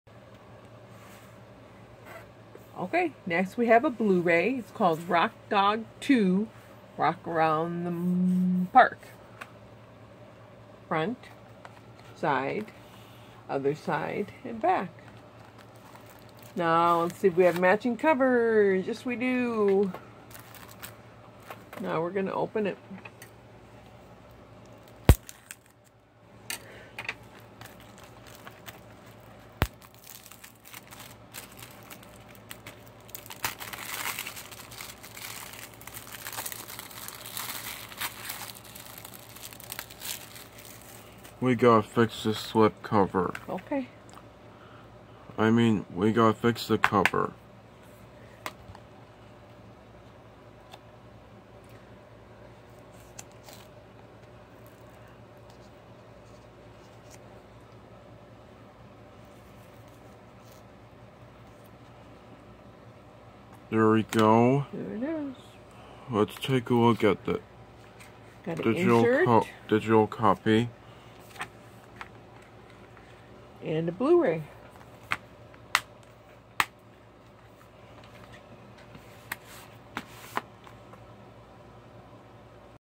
Rock Dog 2: Rock Around the Park Blu-ray Unboxing
Here is an unboxing of Rock Dog 2: Rock Around the Park on Blu-ray that I picked up from Walmart.

Recorded on June 16, 2021 using a Samsung Phone.